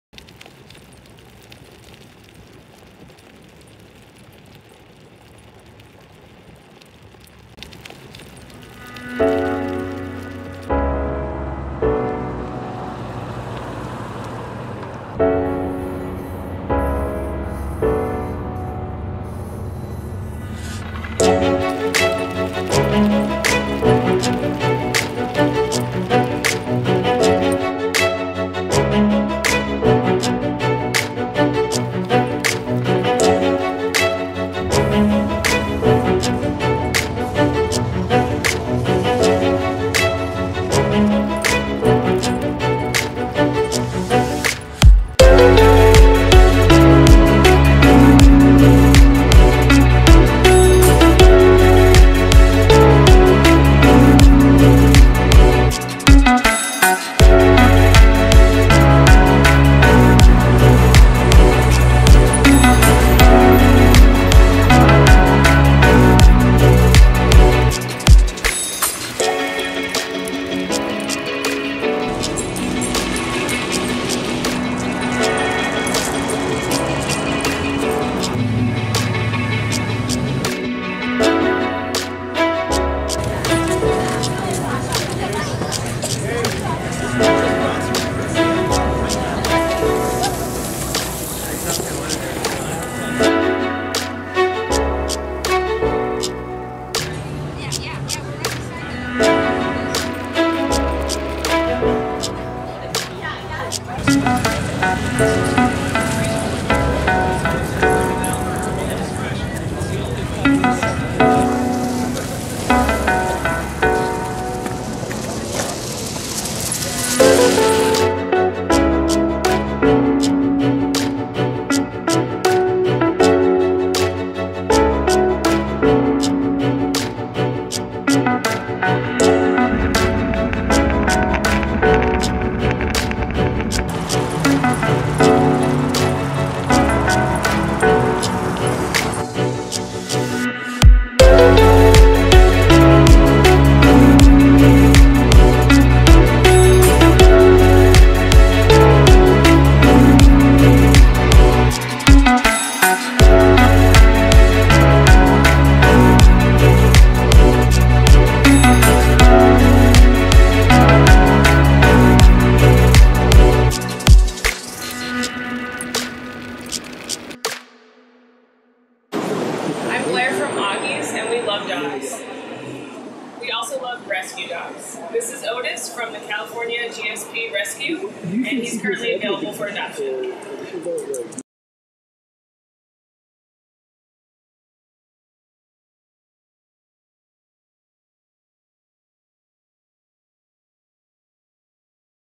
Otis Skate Dog
Otis is an amazing German Shorthaired Pointer that exemplifies the physical qualities of the breed. You won't see another GSP more driven with the athletic ability of Otis. Compared to Michael Jordan, if he could play basketball, he could dunk! On a skateboard, he'll make Tony Hawk drool!

Shout Out to Blair from Augies Coffee for making an appearance!